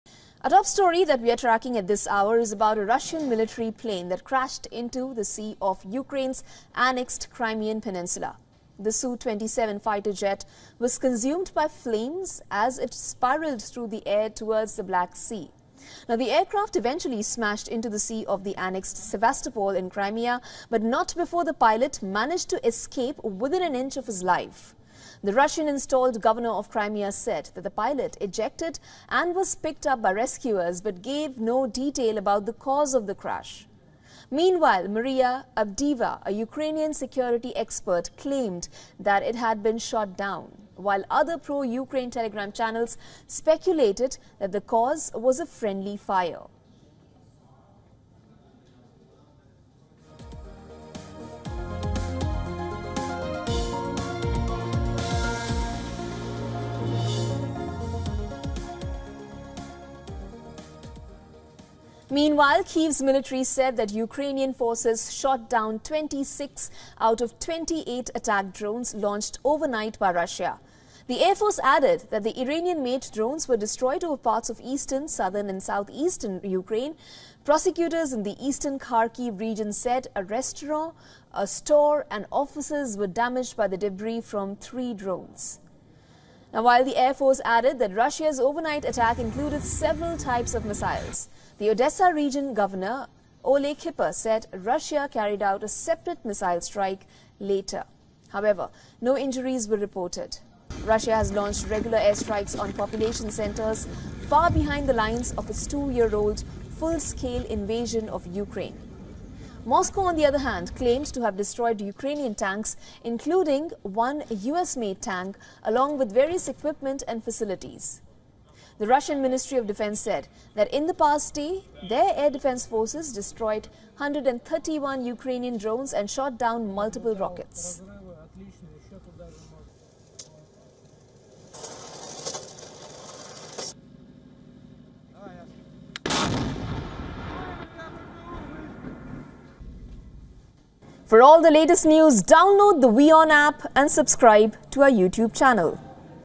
Russia-Ukraine war: Russian SU-27 military jet crashes in Black Sea | Latest News | WION
A Russian military aircraft crashed into the sea on Thursday off the Crimean port of Sevastopol, the Russian-installed governor of the region said. The pilot safely ejected and was picked up by rescuers. Сivilian objects were not damaged, Sevastopol governor Mikhail Razvozhayev said. Watch the video to know more.